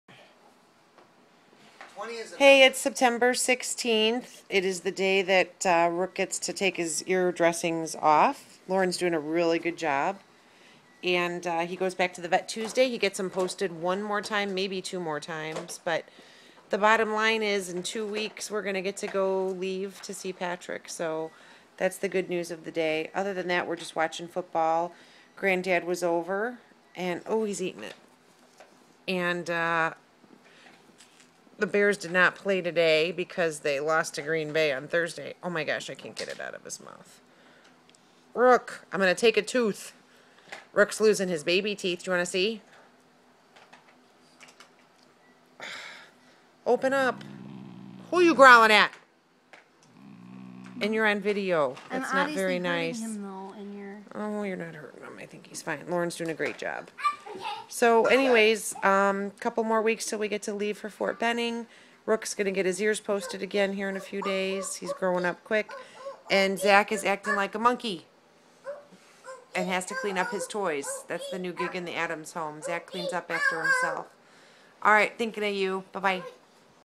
Day 91, Removing posts on Rook, Sept 16
Good doberman puppy getting his ears untaped.